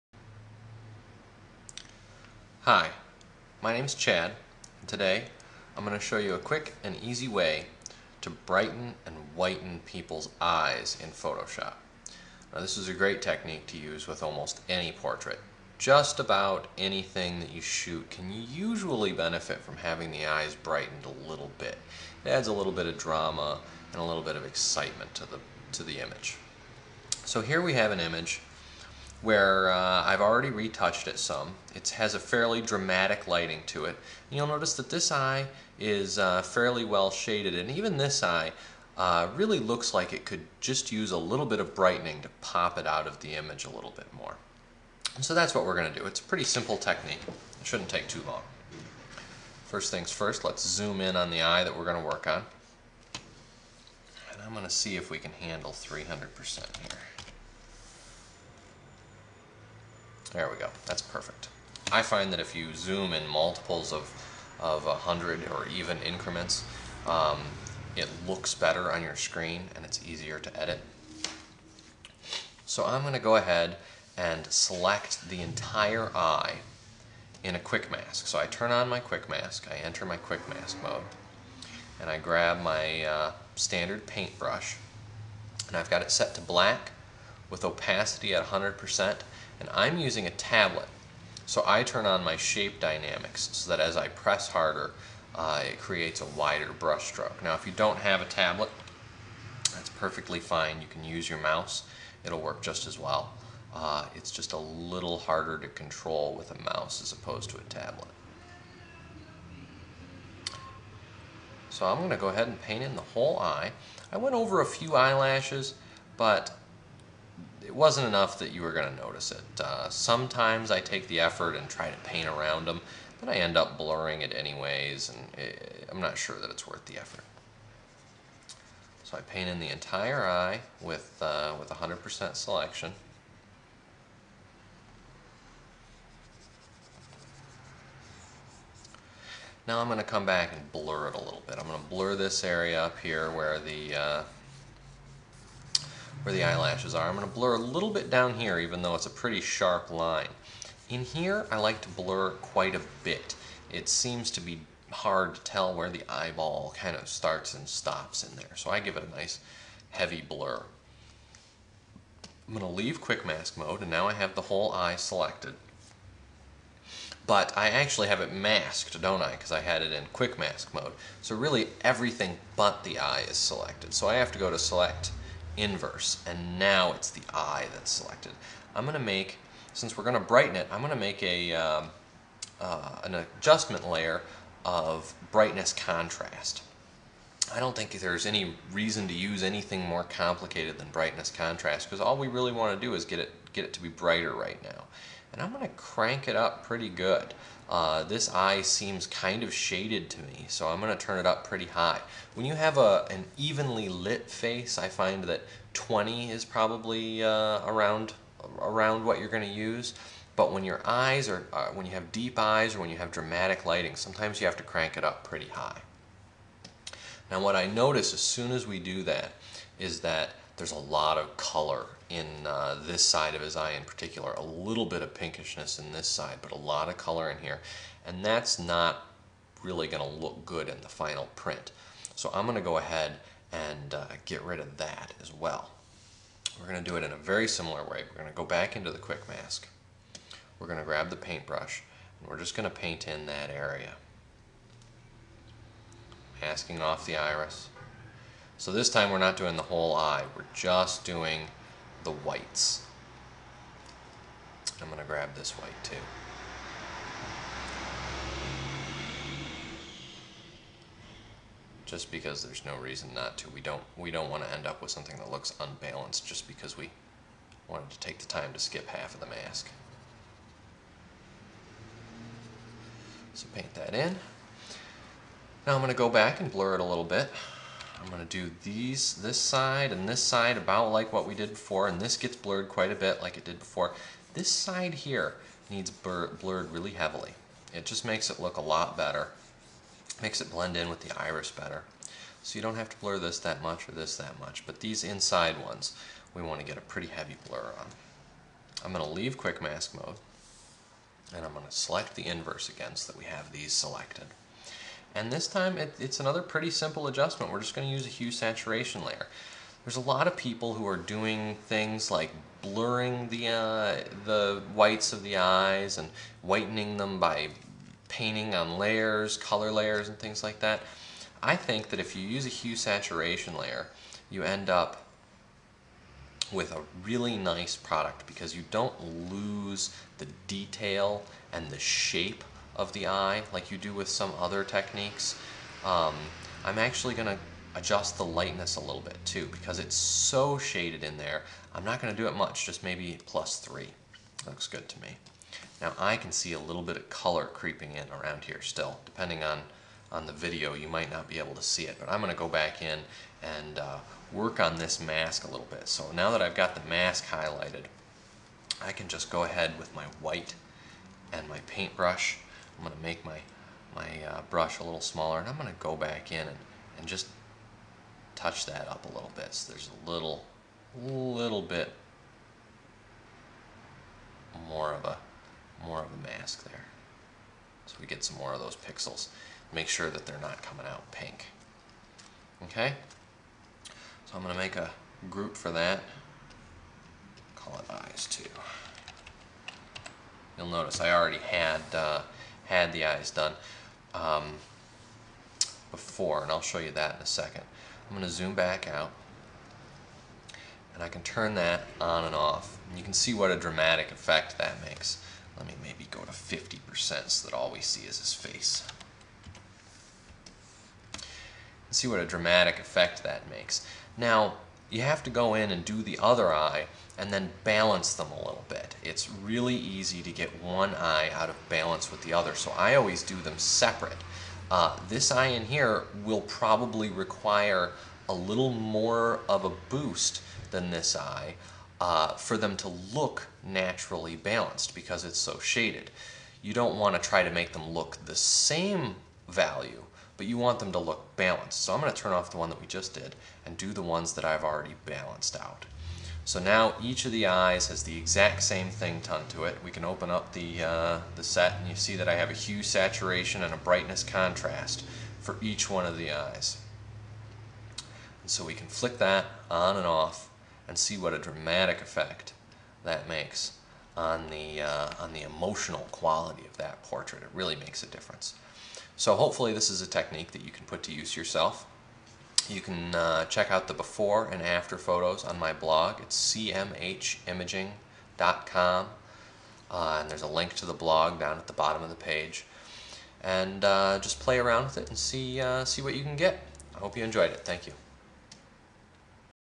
Photoshop Tutorial: "Eyes"
Brighten and whiten eyes for dramatic effect